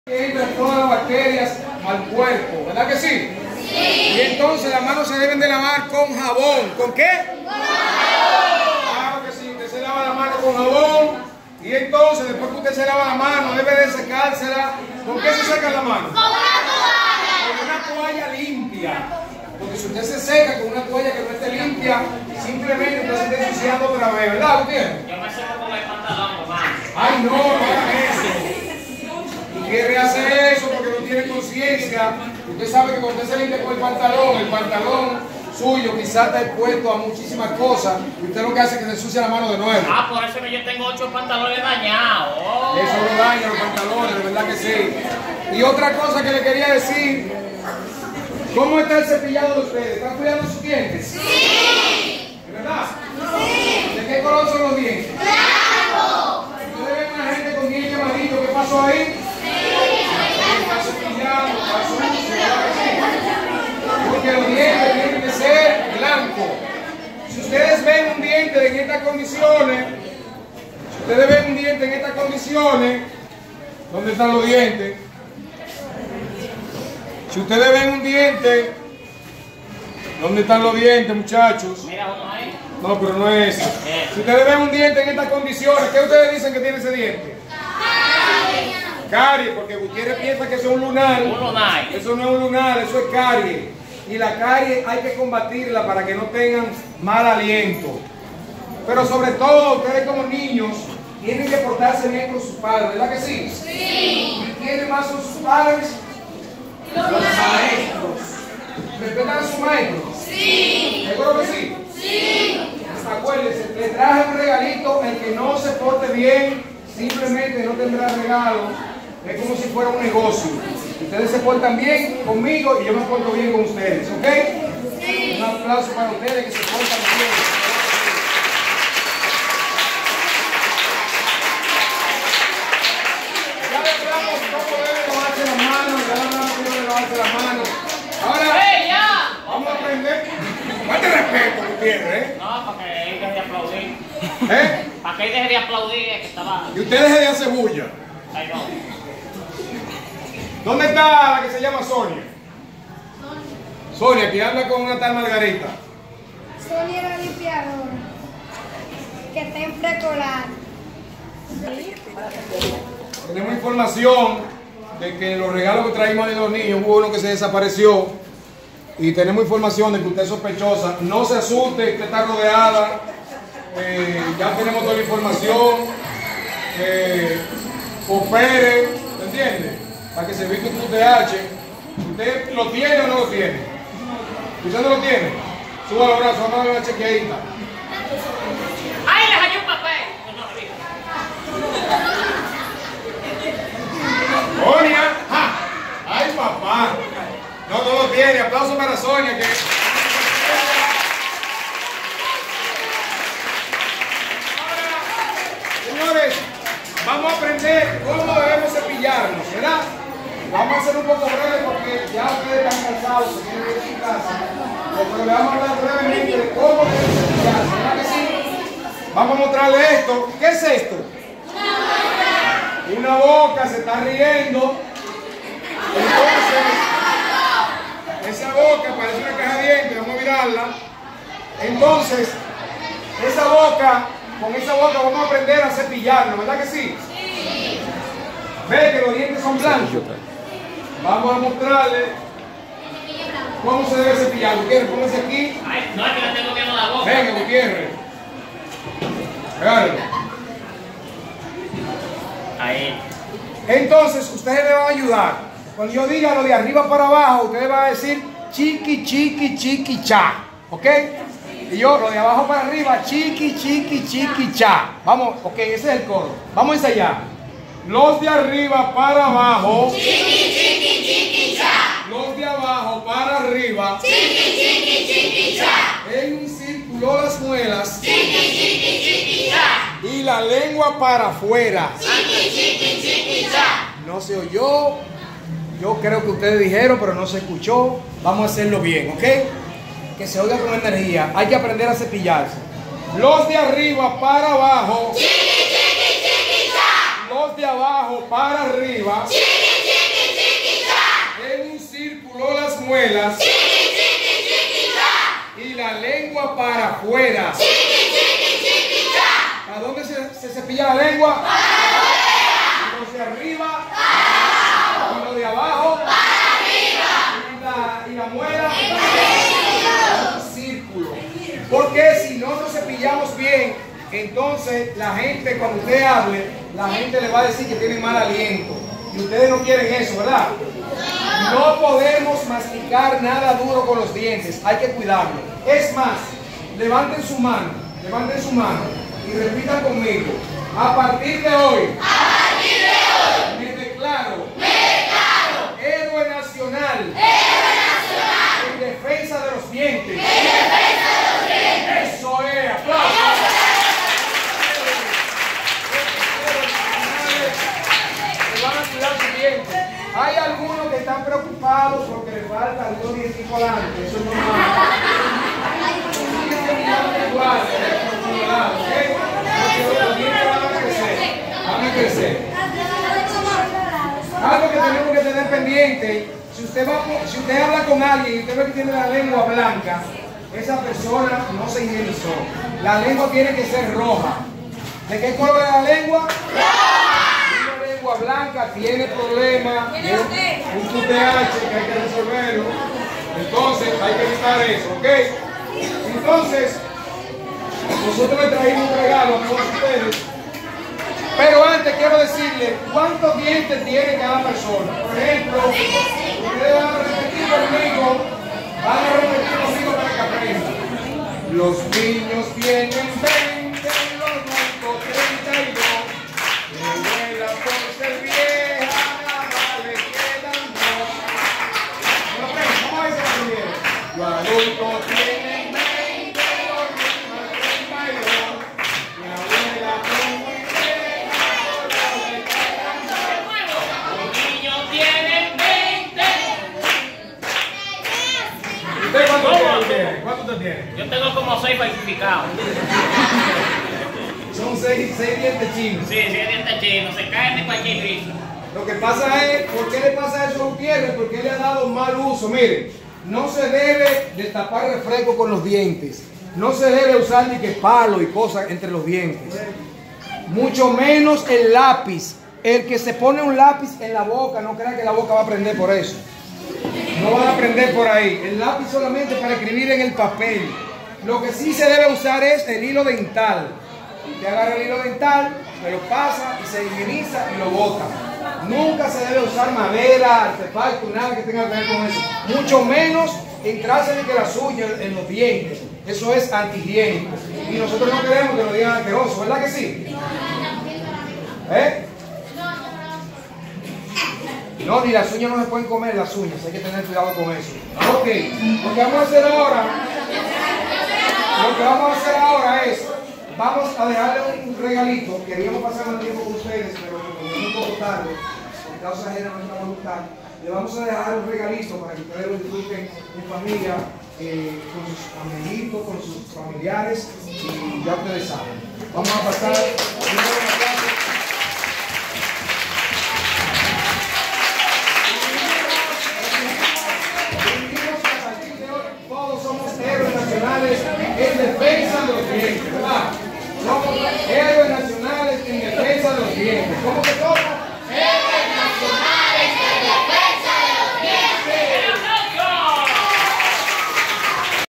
El Comando en escuelas de VillaAltagracia. Orienta a los niños sobre el comportamiento y la higiene
Visitando escuelas de VillaAltagracia